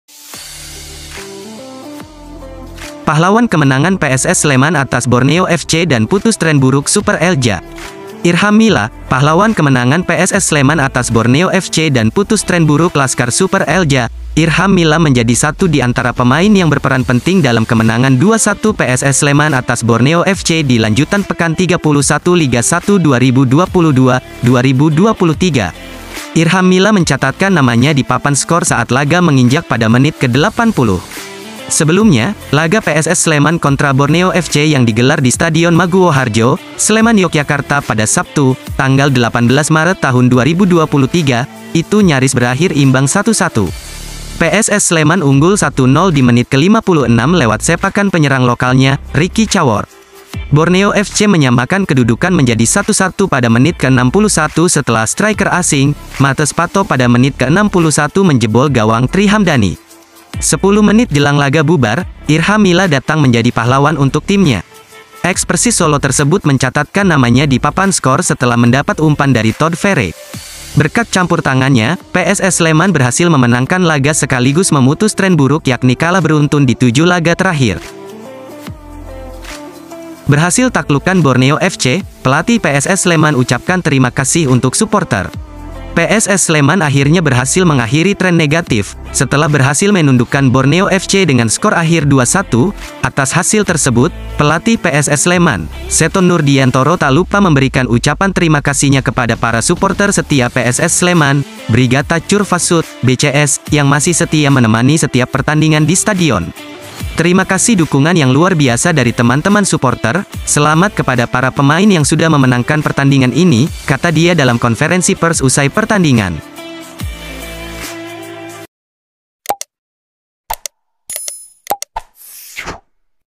PAHLAWAN KEMENANGAN!! Akhirnya PSS Bisa Menang! Seto Nurdiantoro Ucapkan Terima Kasih...
Kumpulan berita terhangat di dunia sepak bola PSS Sleman: Pahlawan Kemenangan PSS Sleman atas Borneo FC dan Putus Tren Buruk Super Elja. Berhasil Taklukkan Borneo FC, Pelatih PSS Sleman Ucapkan Terima Kasih untuk Suporter